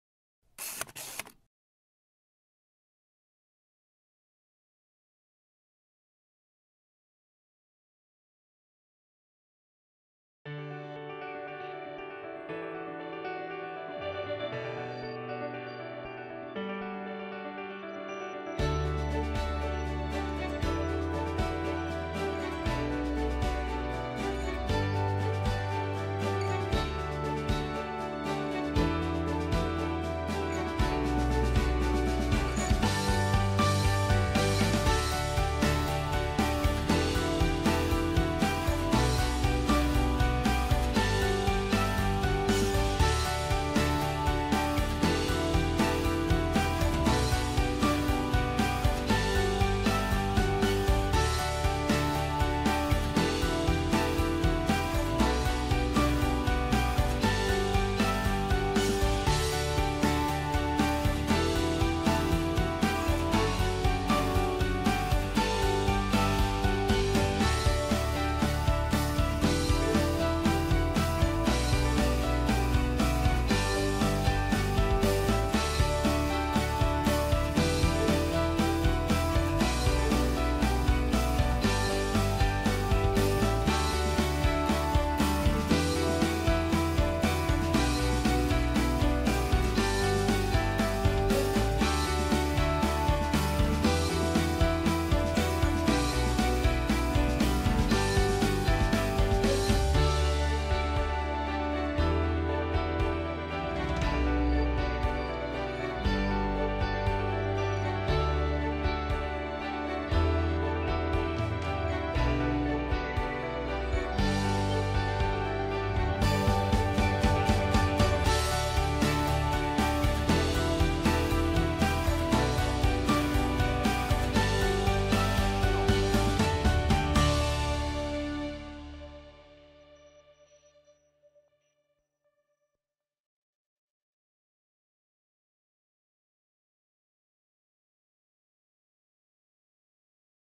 Farmer Membership Day 2017_English version
Siam Quality Starch in Chaiyaphum, the only one tapioca starch factory in Thailand who set up Farmer Membership for Tapioca Root Farmer. 24th Farmer membership day has been set up to return value to our members.